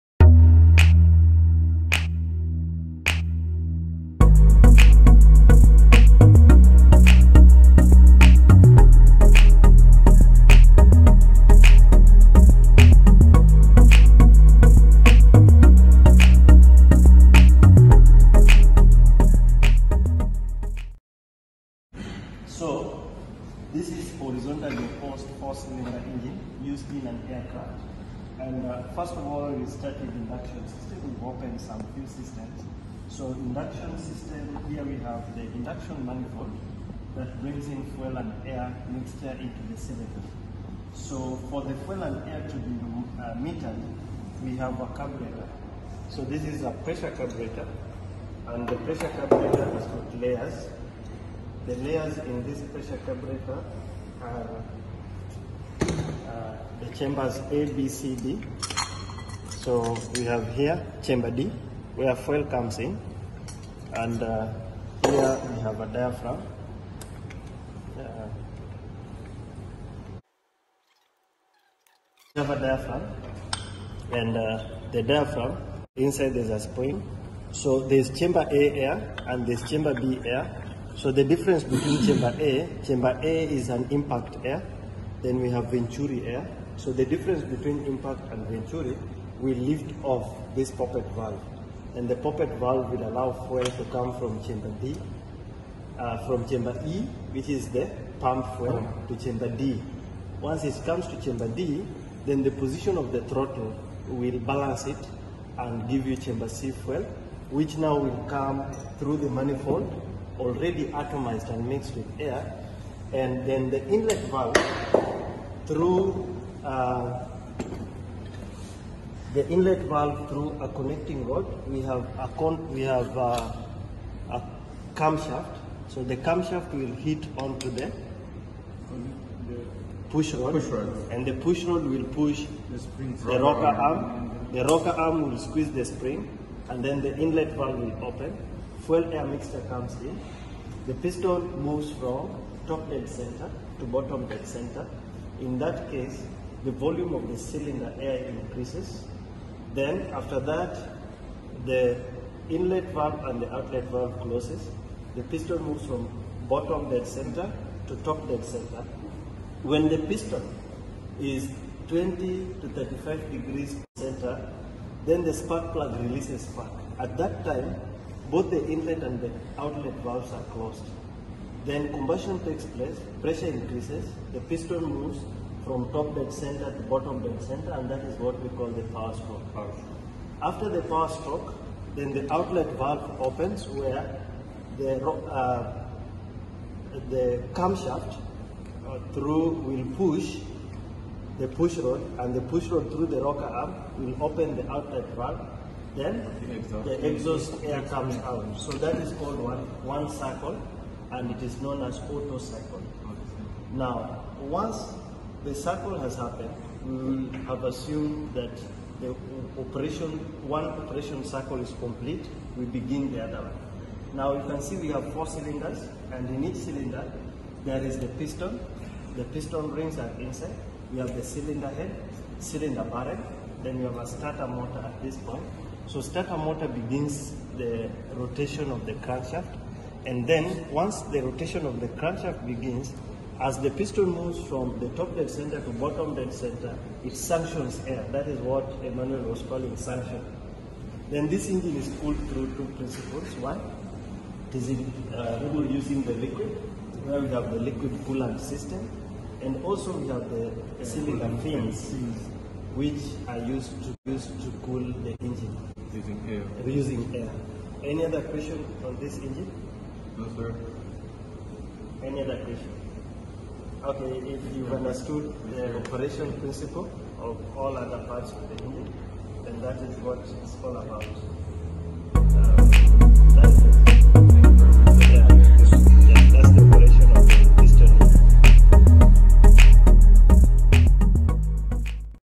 AIRCRAFT PISTON ENGINE, PRINCIPLES OF AIRCRAFT PISTON ENGINE OPERATION.
Full breakdown of the aero-piston engine and the principles of piston engine operation.